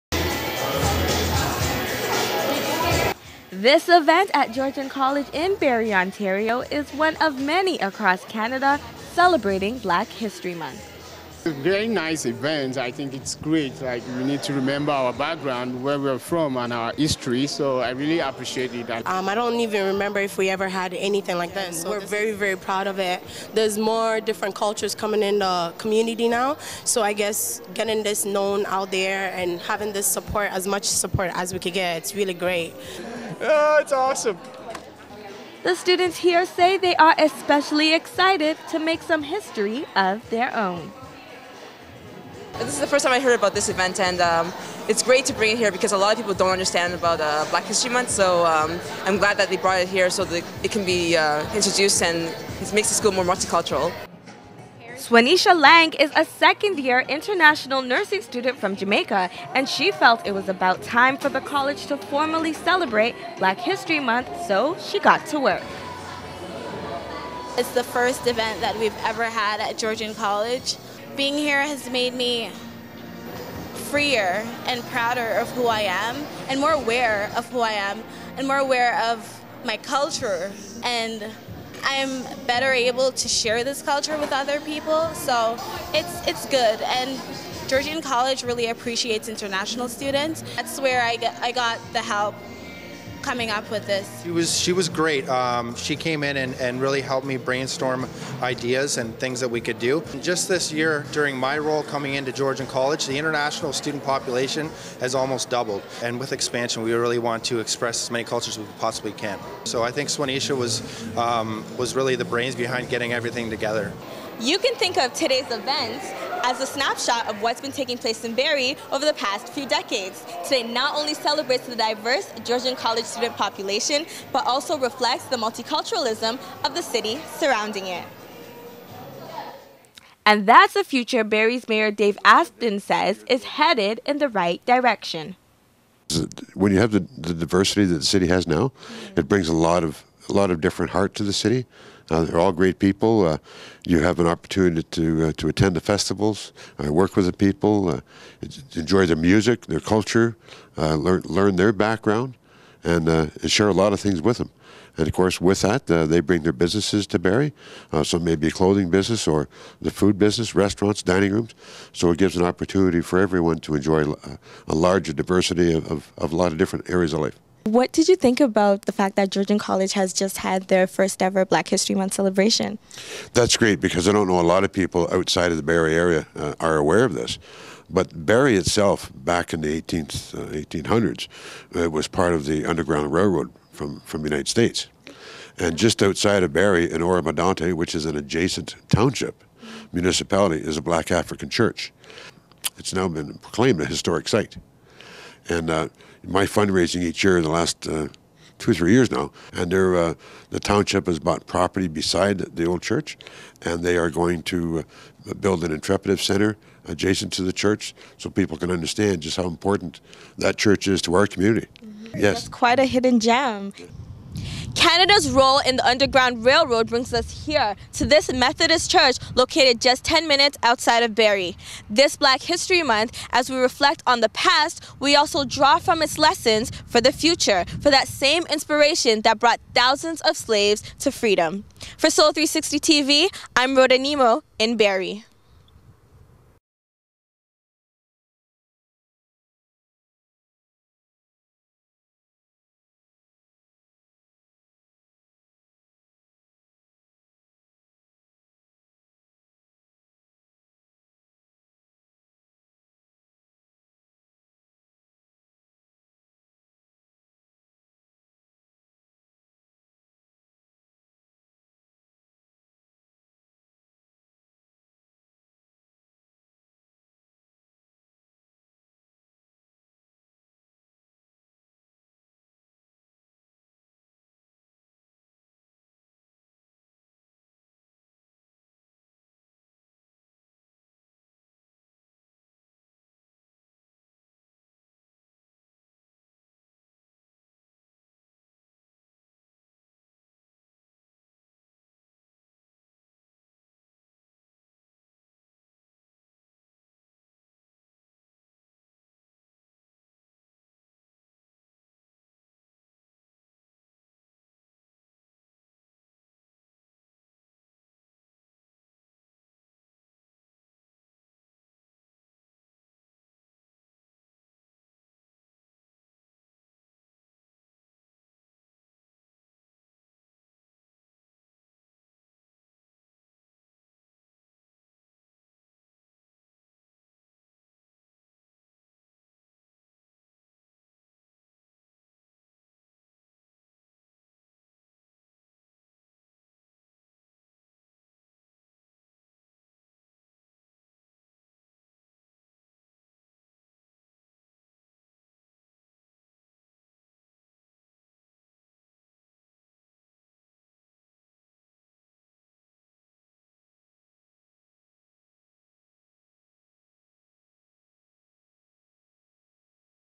Black History in Barrie
Georgian College in Barrie Ontario has their very first Black History Month celebration and Mayor Dave Aspden talks about Barrie's contribution to Canada's Black History